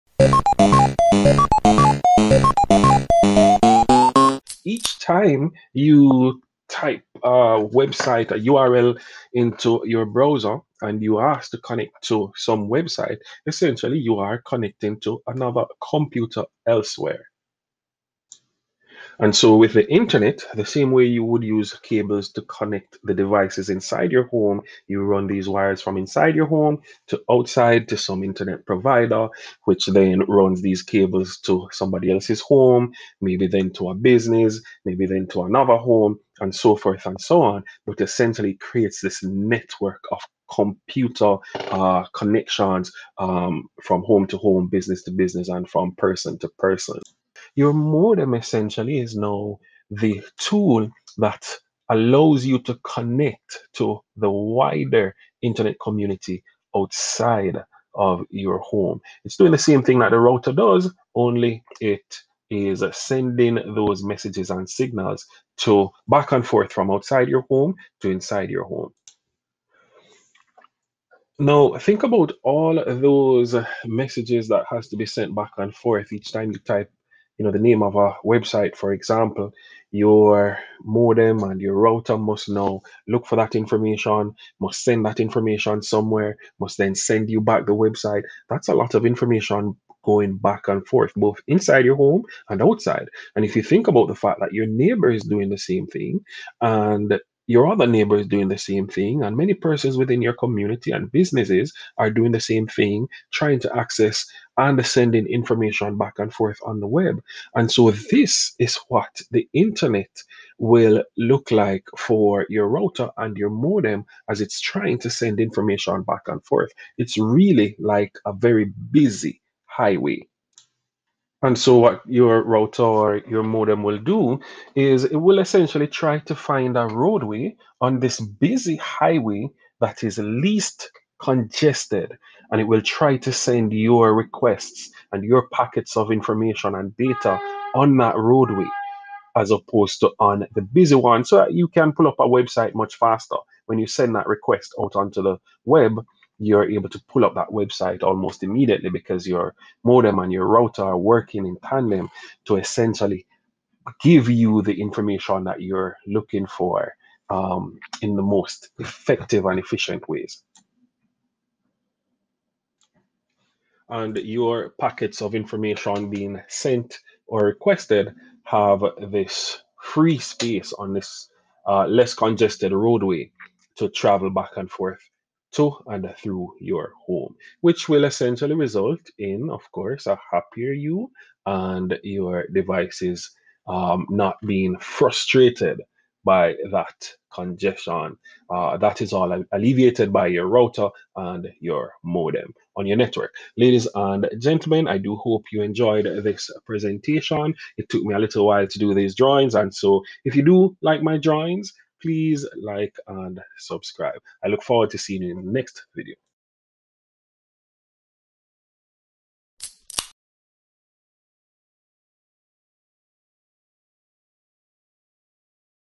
What is a Modem?
A brief description of modems and how they work